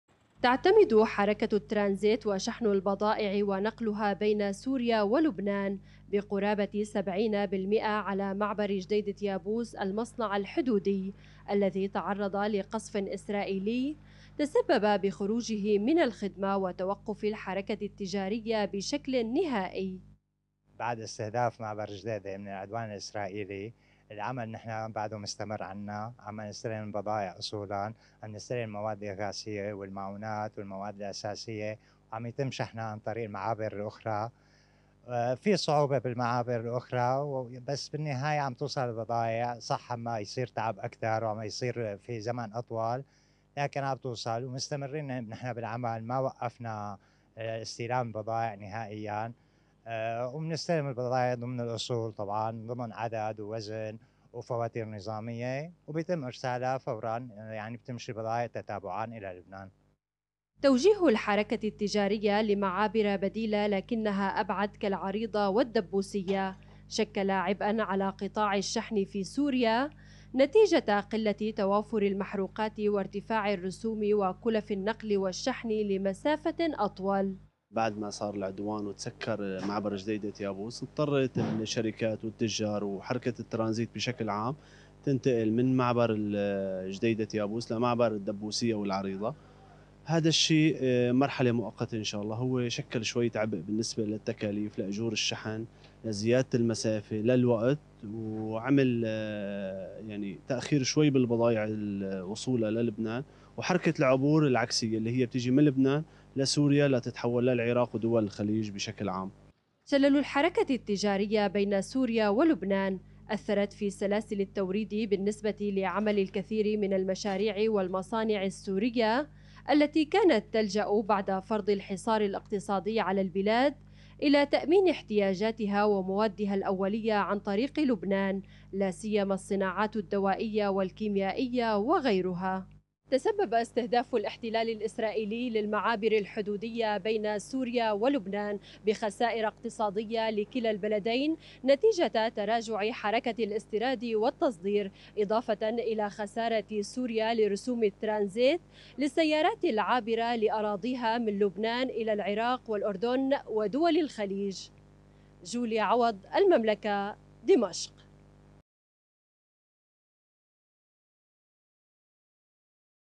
تحويل حركة نقل البضائع بين سوريا ولبنان إلى معابر جديدة
استهداف المعابر الحدودية بين لبنان وسورية تتسبب بخسائر اقتصادية لكلا البلدين نتيجة تراجع حركة الاستيراد والتصدير

ـــــــــــــــــــــــــــــــــــــــــــــــــــــــــ
AlMamlakaTV
Al Mamlaka
أخبار الأردن
أخبار العالم